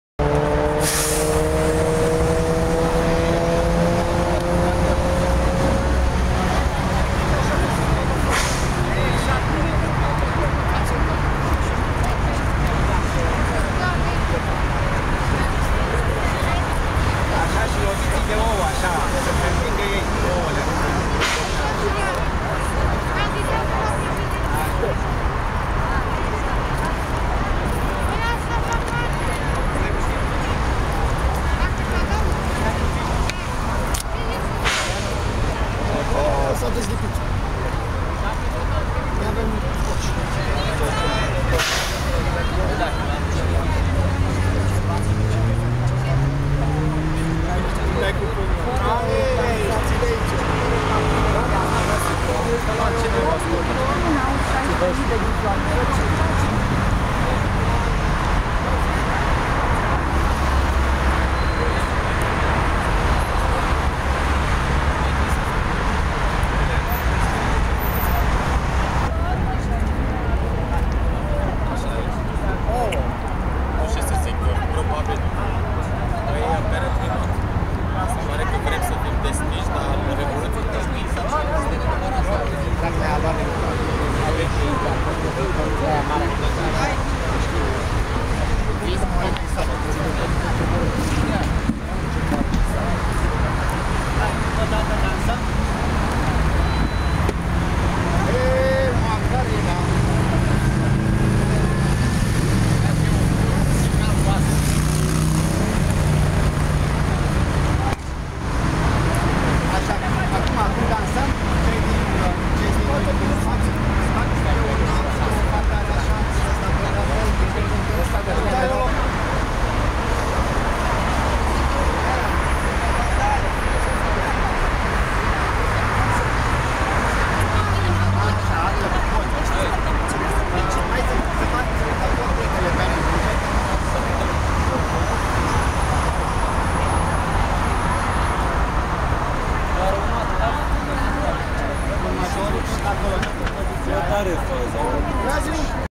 Spectacol de marionete vărgate ]n Piața Victoriei, la Guvern - Curaj.TV
Manifestantii au venit si azi cu sperietori vargate care poarta chipurile lui Dragnea, Grindeanu, Iliescu, Tariceanu. Dar au aparut si personaje ca Basescu, Firea, Vasilescu si alte femei care se gudura pe linga liderul PSD